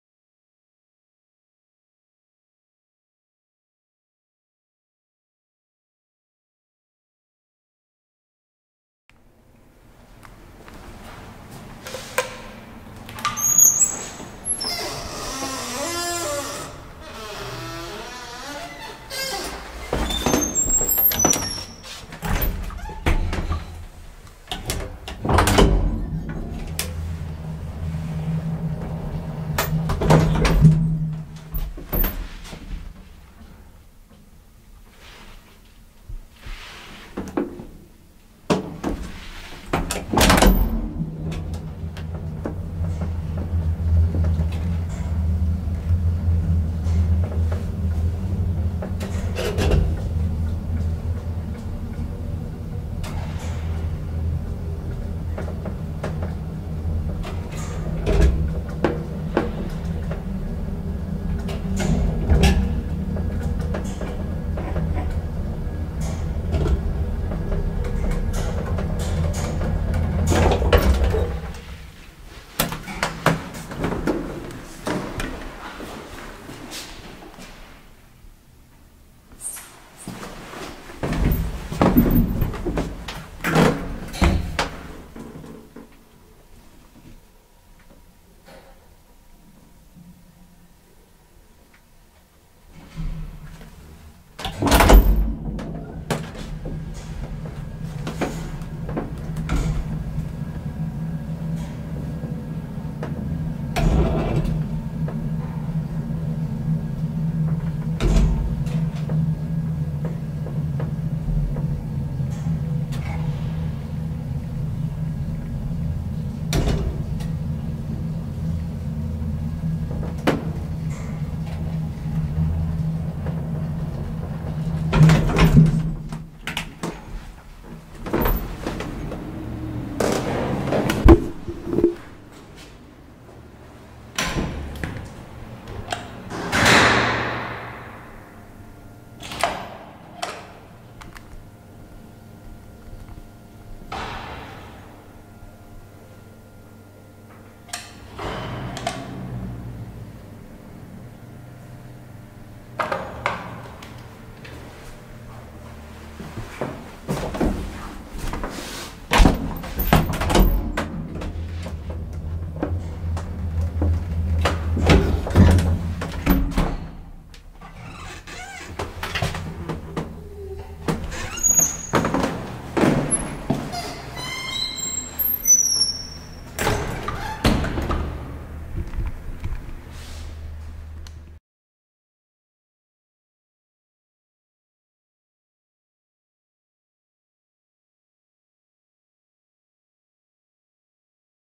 1956 ASEA Traction elevator @ Brahegatan 30, Stockholm, Sweden!
ELEVATOR INFORMATION!

Manufacturer/Tillverkare: ASEA
Model/Modell:
Installation year/Byggår: 1956
Serial Number/Tillverkningsnummer:
Capacity/Maxvikt: 320Kg - 4 Persons
Floors/Antal våningar: 8 = K B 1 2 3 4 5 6

Location/Adress: Brahegatan 30, 114 37 Stockholm (Östermalm) SWEDEN!
Camera/Kamera: Panasonic HC-V720!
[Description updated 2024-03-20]